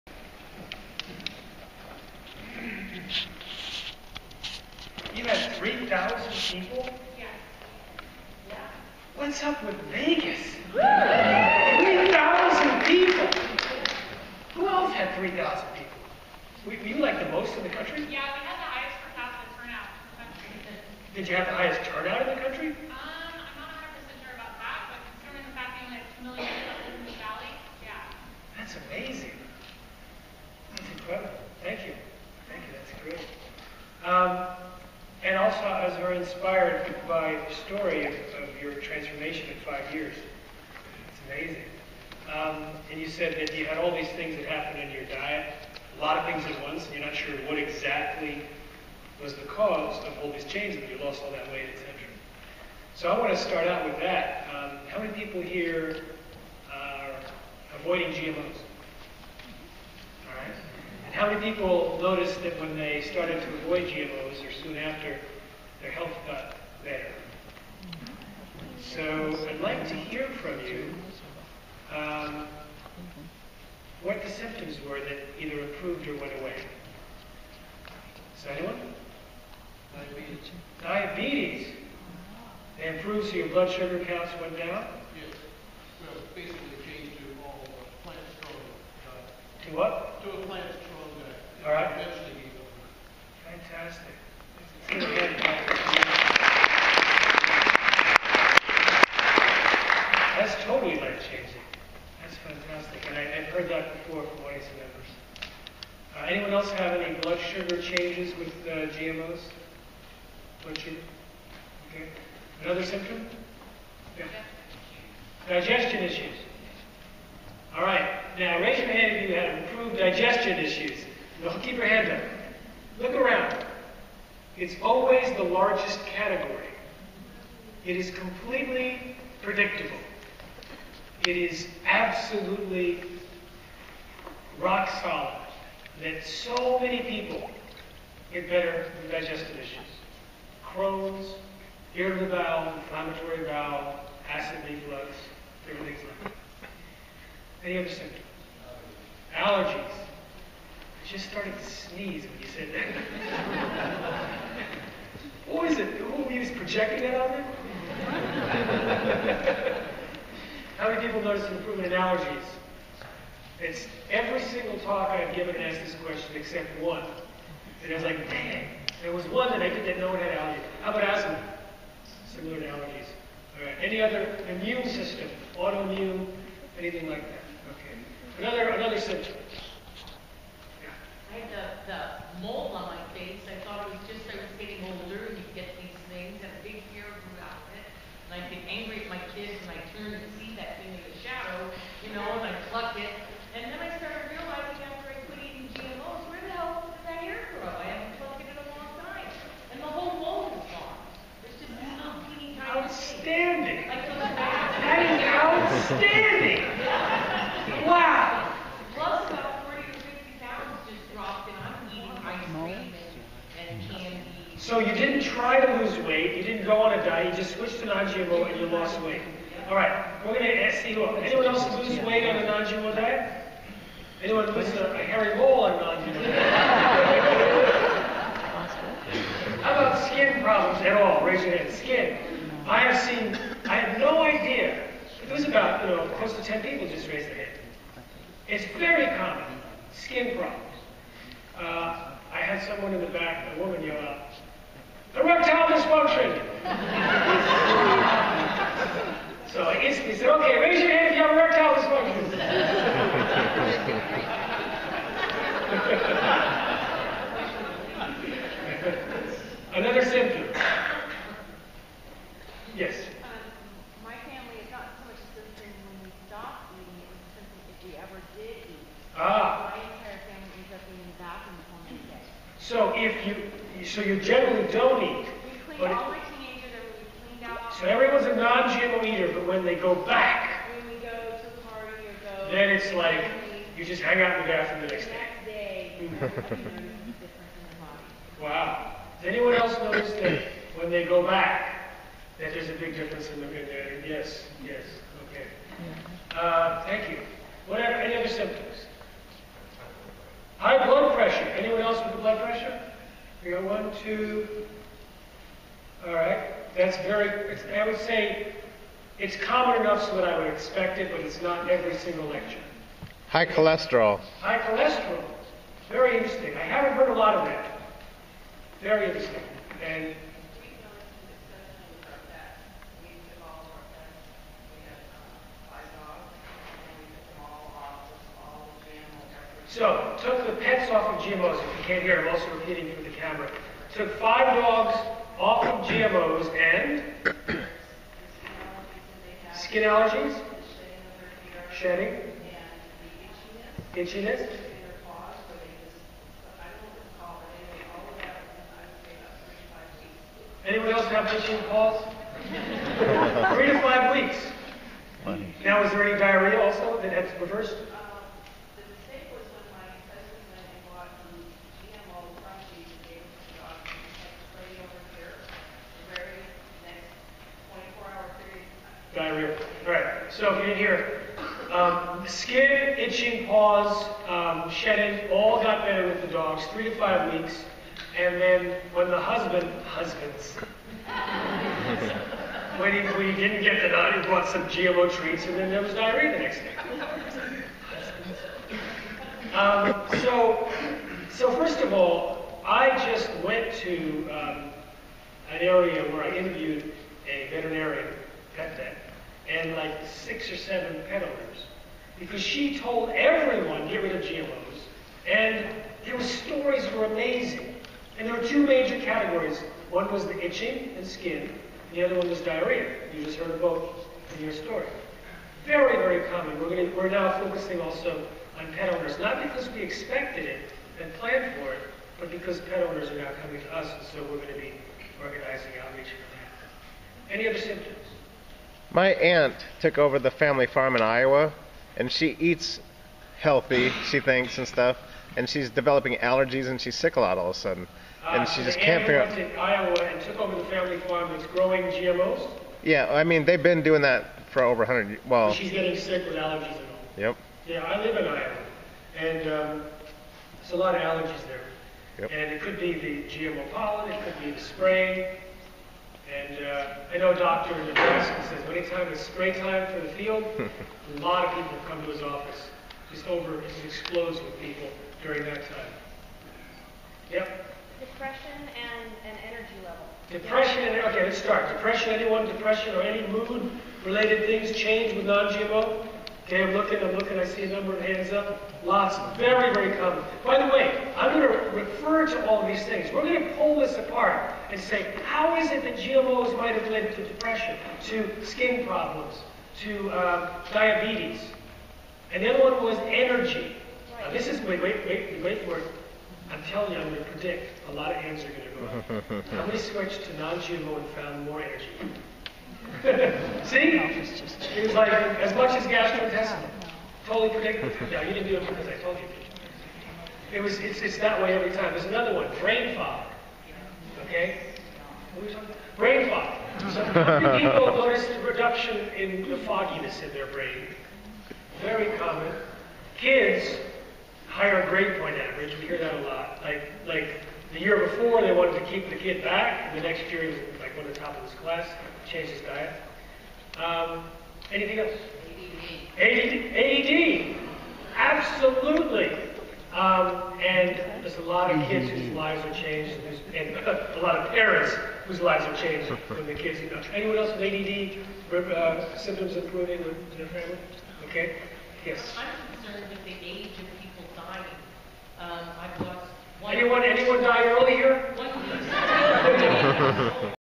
Genetic Roulette speach
An incredible talk from the writer of the movie "Genetic Roulette" about how Monsanto and the big food companies are poisoning us with our foods and the general public has absolutely no idea!